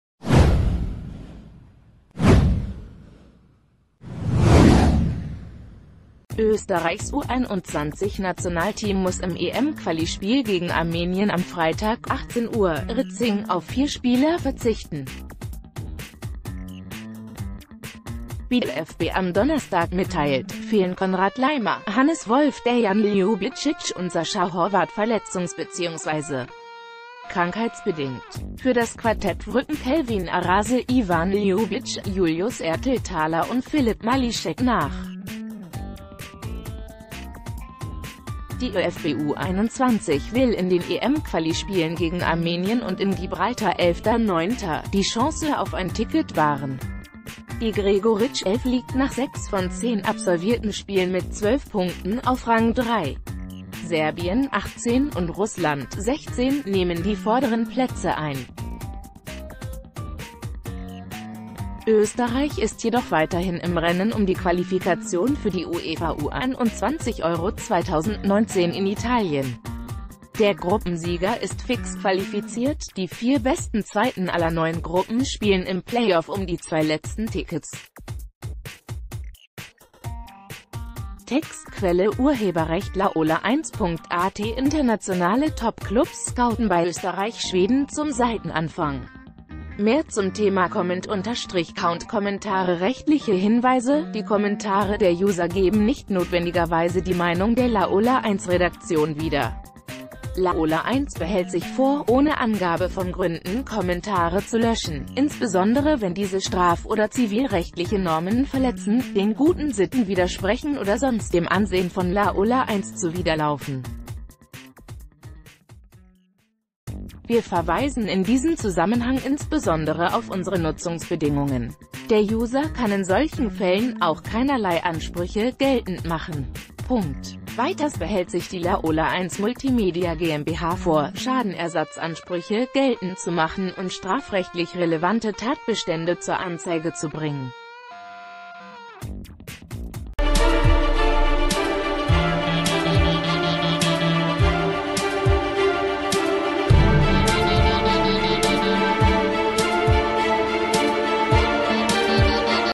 Vier Ausfälle beim ÖFB-U21-Team
Österreichs U21-Nationalteam muss im EM-Quali-Spiel gegen Armenien am Freitag (18 Uhr/Ritzing) auf vier Spieler verzichten. Wie der ÖFB am Donnerstag mitteilt, fehlen Konrad Laimer, Hannes Wolf, Dejan Ljubicic und Sascha Horvath verletzungs- bzw. krankheitsbedingt. Für das Quartett rücken Kelvin Arase, Ivan Ljubic, Julius Ertlthaler und Philipp Malicsek nach. Die ÖFB-U21 will in den EM-Quali-Spielen gegen Armenien und in Gibraltar (11.9.) die Chance auf ein Ticket wahren. Die Gregoritsch-Elf lie...